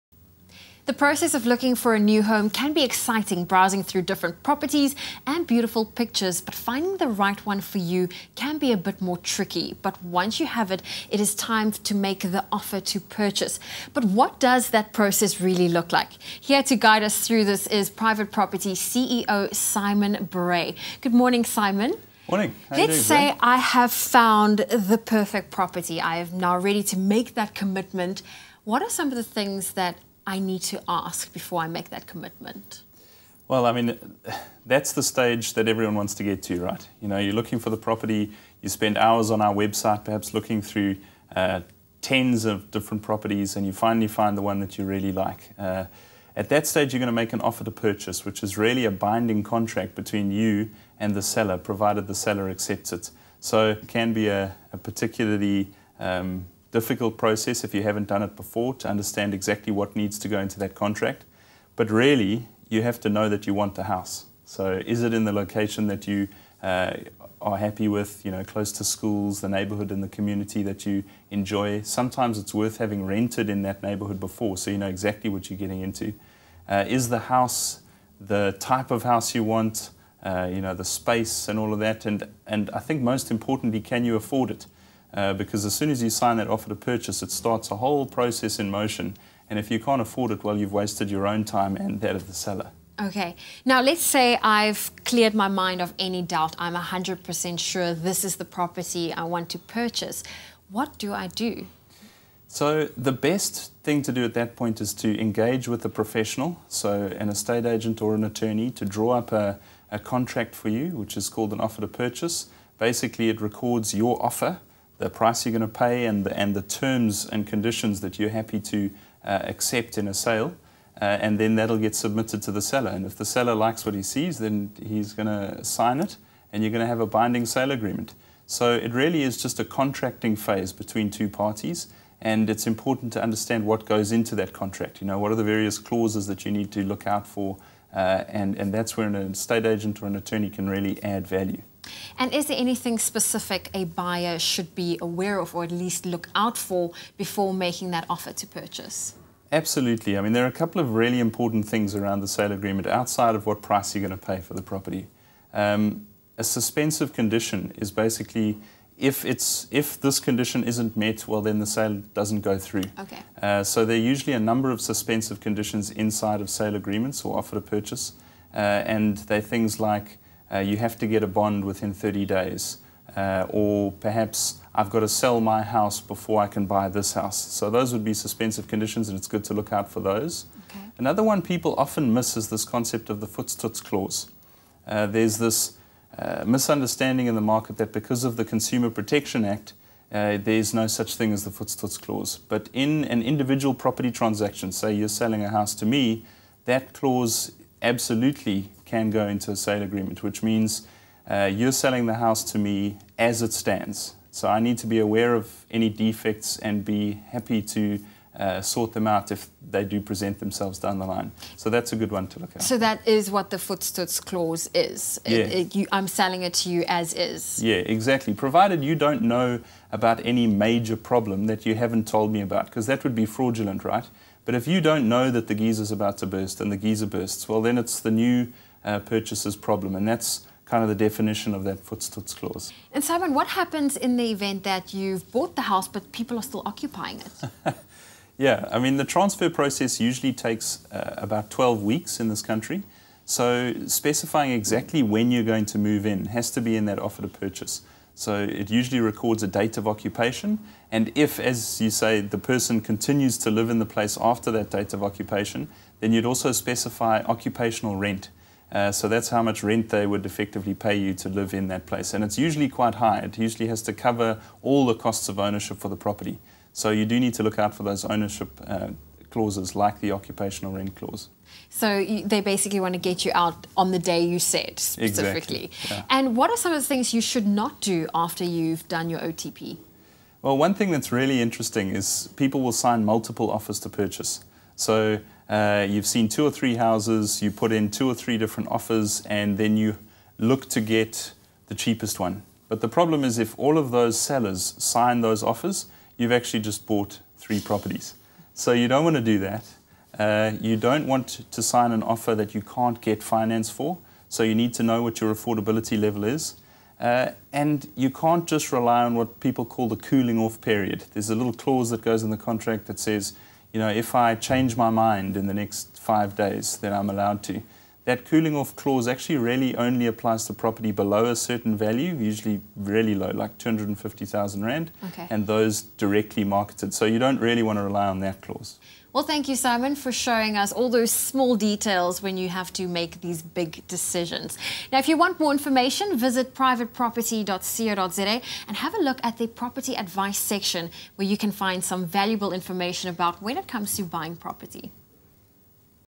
Making the offer to purchase
VIDEO TRANSCRIPT:

Reporter: Let's say I have found the perfect property. I am now ready to make that commitment. What are some of the things that I need to ask before I make that commitment?

Simon Bray: You're looking for the property, and you finally find the one that you really like. At that stage, you're going to make an offer to purchase, which is really a binding contract between yo, and the seller, provided the seller accepts it. So it can be a particularly difficult process, if you haven't done it before, to understand exactly what needs to go into that contract but really you have to know that you want the house. So is it in the location that you're happy with? Close to schools, the neighbourhood and the community that you enjoy. Sometimes it's worth having rented in that neighbourhood before so you know exactly what you're getting into. Is the house the type of house you want? The space and all of that, and I think most importantly can you afford it, because as soon as you sign that offer to purchase it starts a whole process in motion. And if you can't afford it, well you've wasted your own time and that of the seller.

Reporter: Okay, now let's say I've cleared my mind of any doubt, I'm a 100% sure this is the property I want to purchase, what do I do?

Simon Bray: The best thing to do at that point is to engage with a professional, so an estate agent or an attorney, to draw up a contract for you which is called an offer to purchase. And then it'll get submitted to the seller, and if the seller likes what he sees then he's going to sign it, and you're going to have a binding sale agreement. So it really is just a contracting phase between two parties, and it's important to understand what goes into that contract, what are the various clauses that you need to look out for and that's where an estate agent agent or an attorney can really add value.

Reporter: And is there anything specific a buyer should be aware of, or at least look out for, before making that offer to purchase?

Simon Bray: Absolutely. A suspensive condition is basically if this condition isn't met well then the sale doesn't go through. So there's usually a number of suspensive conditions inside of sale agreements or offer to purchase, and there are things like you have to get a bond within 30 days, or perhaps I've got to sell my house before I can buy this house. So those would be suspensive conditions, and it's good to look out for those. Another one people often miss is this concept of the Voetstoots clause. There's this misunderstanding in the market that because of the Consumer Protection Act, there's no such thing as the Voetstoots clause. But in an individual property transaction, say you're selling a house to me, that clause absolutely can go into a sale agreement which means you're selling the house to me as it stands. So I need to be aware of any defects and be happy to sort them out if they do present themselves down the line. So that's a good one to look out for. Provided you don't know about any major problem that you haven't told me about, because that would be fraudulent, right?

Reporter: Simon what happens in the event that you've bought the house but people are still occupying it?

Simon Bray: The transfer process usually takes about 12 weeks in this country. So specifying exactly when you're going to move in has to be in that Offer to Purchase. So it usually records a date of occupation, and if, as you say, the person continues to live in the place after that date of occupation, then you'd also specify occupational rent. So that's how much rent they would effectively pay you to live in that place. It's usually quite high, it usually has to cover all the costs of ownership for the property. So you do need to look out for those ownership clauses, like the occupational rent clause.

Reporter: And what are some of those things you should not do after you've done your OTP?

Simon Bray: One thing that's really interesting is people will sign multiple offers to purchase. You've seen two or three houses, you've put in two or three different offers, and then you look to get the cheapest one. But the problem is if all of those sellers sign those offers, you've actually just bought three properties. So you don't want to do that. You don't want to sign an offer that you can't get finance for, so you need to know what your affordability level is. And you can't just rely on what people call the "cooling off period". There's a little clause that goes in the contract that says, "If I change my mind in the next five days, then I'm allowed to." That cooling off clause actually really only applies to property below a certain value, usually really low, like R250,000. And those directly marketed. So you don't really want to rely on that clause.